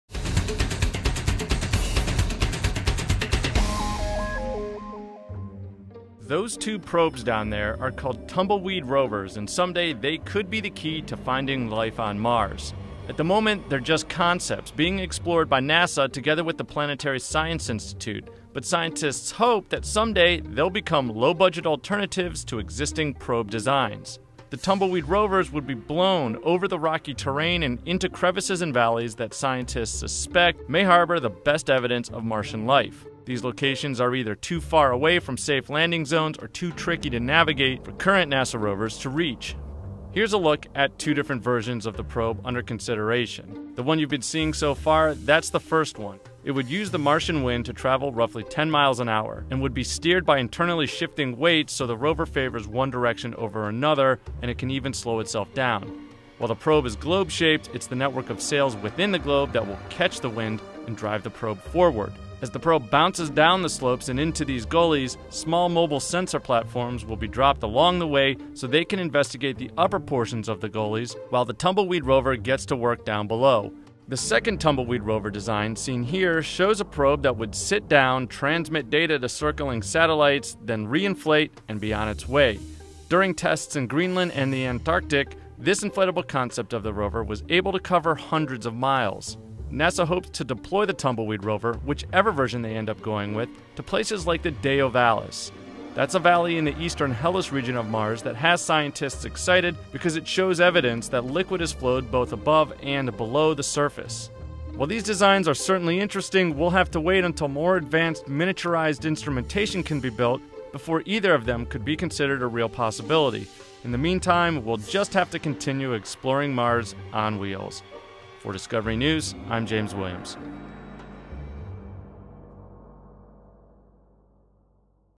Tumbleweed Rovers Could Explore Mars
New concepts for Mars-probing rovers would use Martian wind to move around the planet. James Williams gets a look at two of the designs.